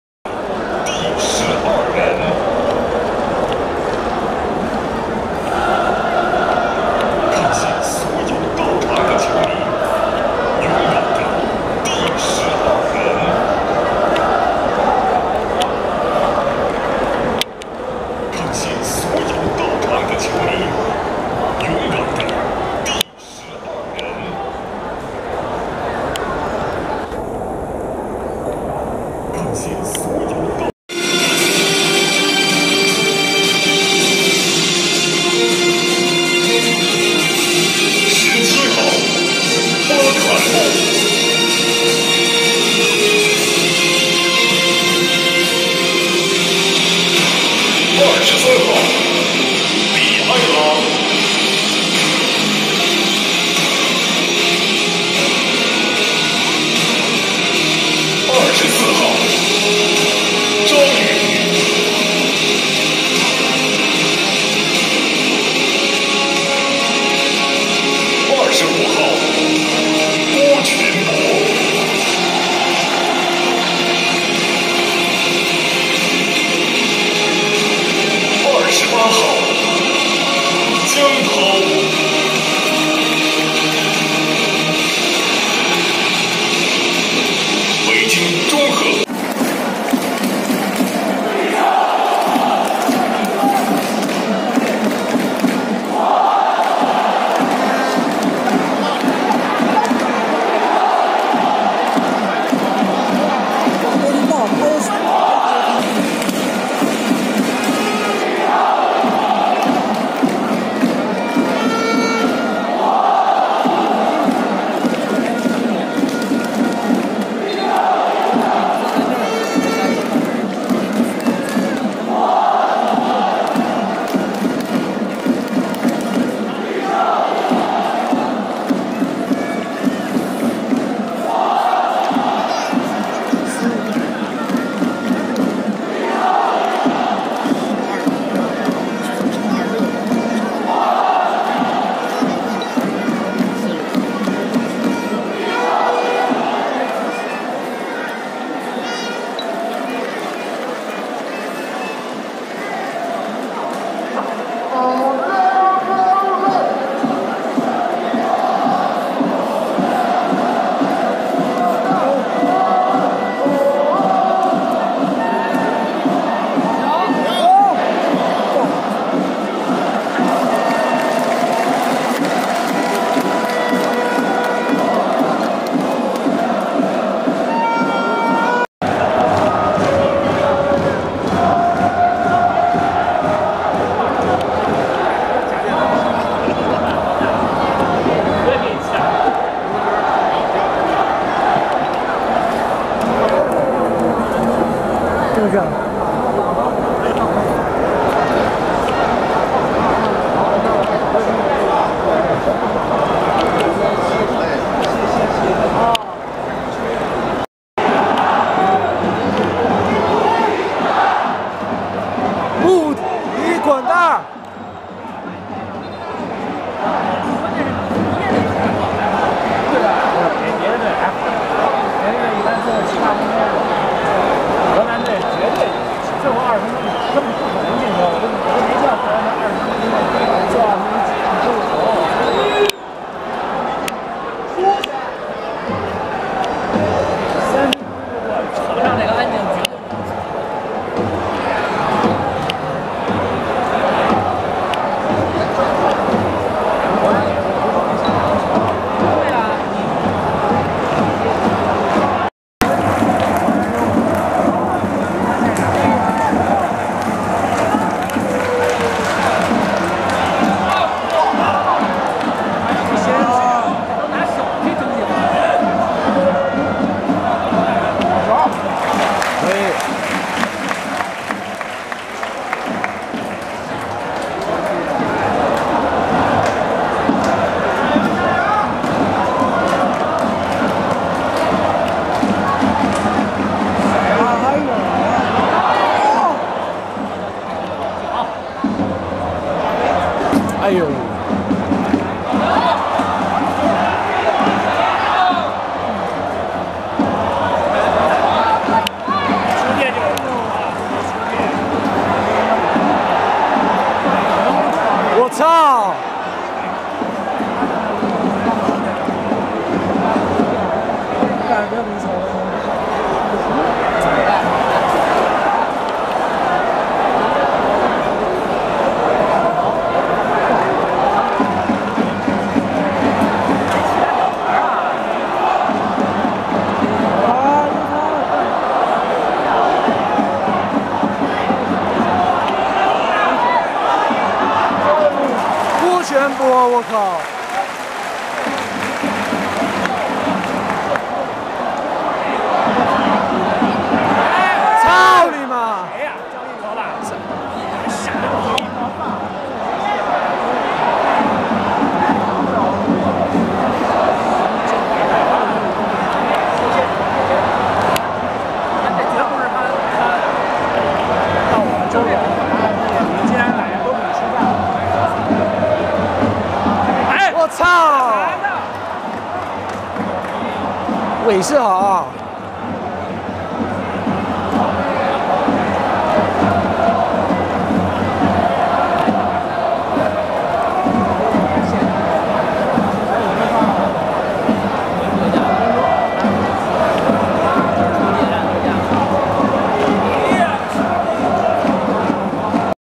北京國安2:1河南建業-比賽花絮合輯 河南遠征軍賽前助威 國安球迷喊抓賊 Ultras Henan's away match in Beijing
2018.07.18 (WED) 19:35 Kick Off

2018 China Super League MatchDay 12

Beijing Guoan 北京國安 (Green)
2:1
Henan Jianye 河南建業 (Red)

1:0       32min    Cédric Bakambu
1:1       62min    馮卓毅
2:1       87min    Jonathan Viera

Venue: Worker Stadium (Beijing,CHINA)
Attendance:40076/66161